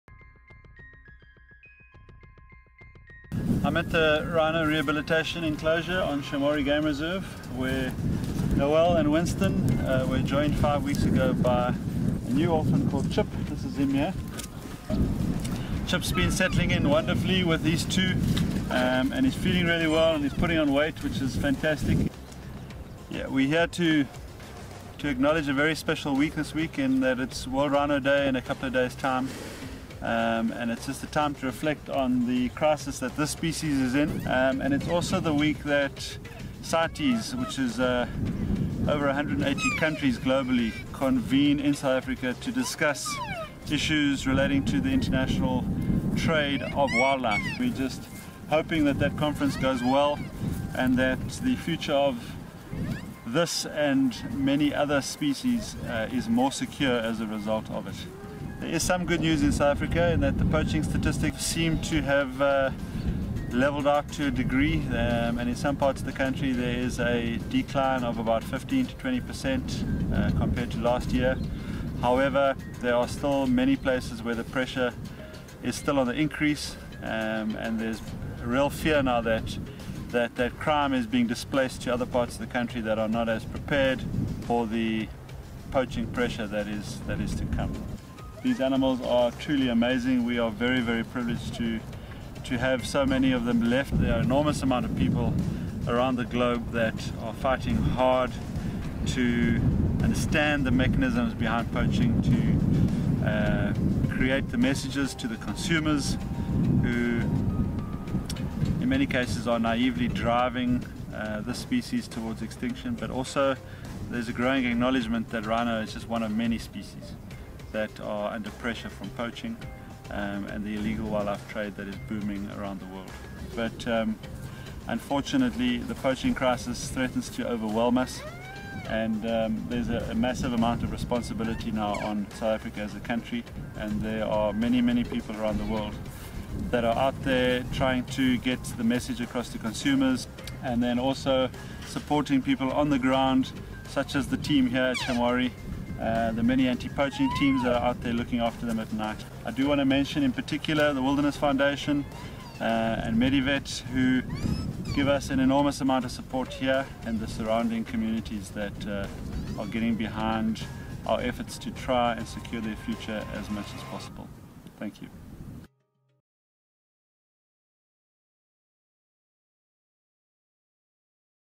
Medivet Rhino - World Rhino Day 2016
In light of World Rhino Day,  here is a short video from Dr William Fowlds on the positive developments in 2016…